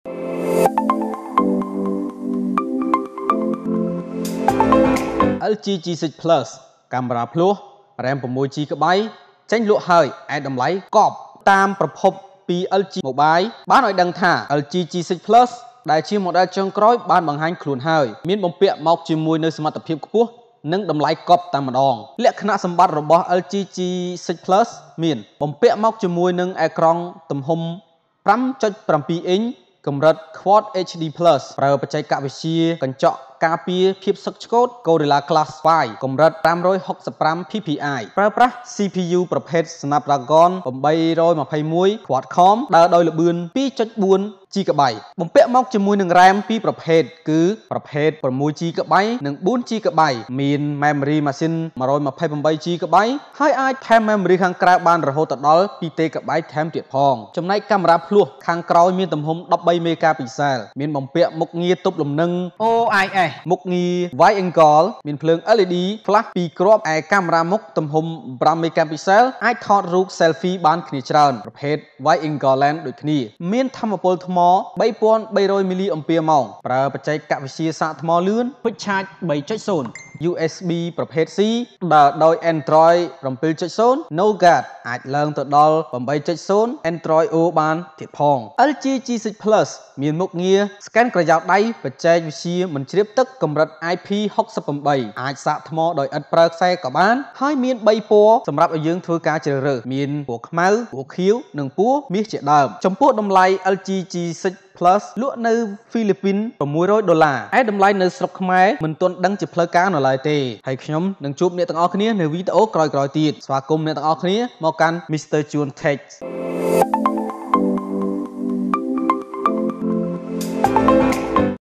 Review LG G6 Plus | LG G6 plus Cambodia [speak khmer] lg khmer
This video show you about LG G6 plus speak khmer . good smart phone 2018.
2 Honda CB1000R 2018 and Cafe Racer | Neo Sports Cafe 2018 New | Review Honda CB1000R 2018
Feature LG G6 +
- screen 5.7 QHD+
-  Gorilla Glass 5 565ppi
- CPU Snapdragon 821 Quad-core, speed 2.4 gb
- Camera back 13 MP wide angle
- OIS LED Flash